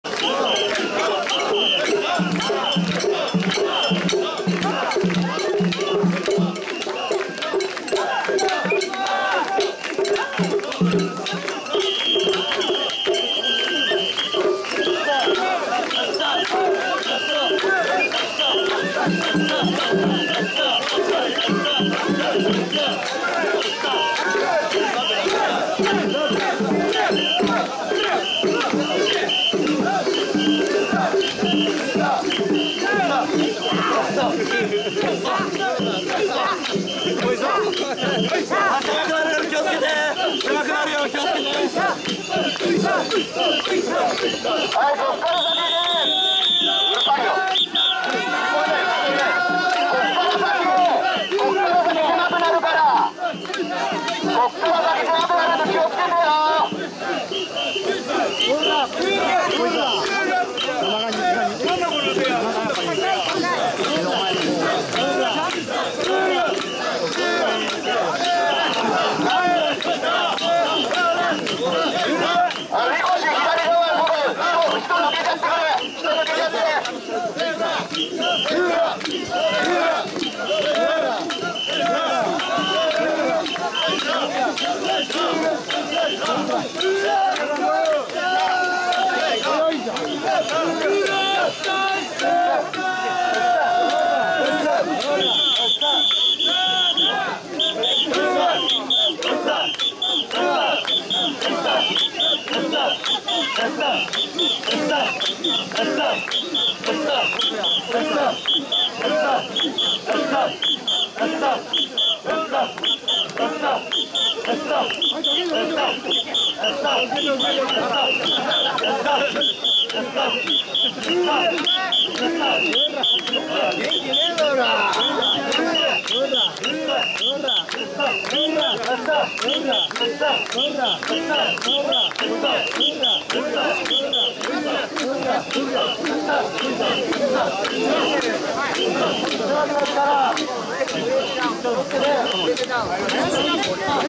平成28年 立川 諏訪神社 例大祭 宮出し「みよし會」神輿半端無い渡御。
【参加団体】
1-　立川花睦神輿　台座3尺（91cm）
2- 立川北睦神輿
3- 彩心連合「東部心睦神輿 台座三尺五寸（106cm）大型神輿です。 蕨手も異様に大きいのです。
4- 葱花の鳳輦神輿.
5- 諏訪飛鳳会神輿は台座三尺一寸（95cm）